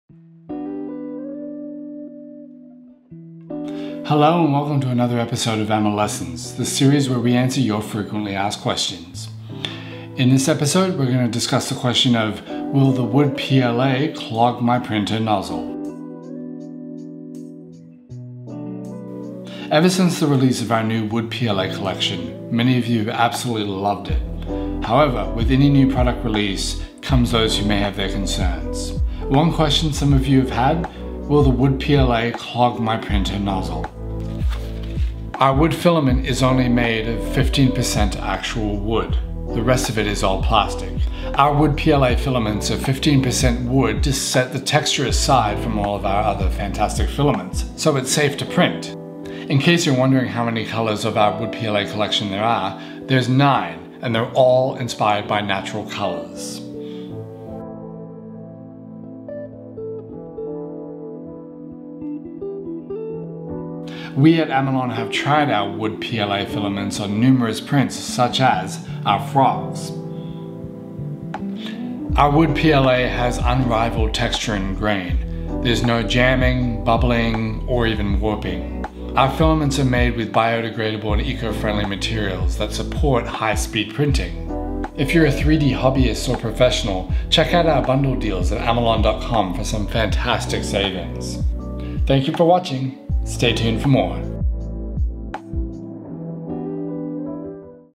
Amolessons: Will the Wood PLA clog my printer?
In this episode of "Amolessons", we discuss the question of "Will the Wood PLA, clog my printer?". We also explore the the numerous natural colors of the collection.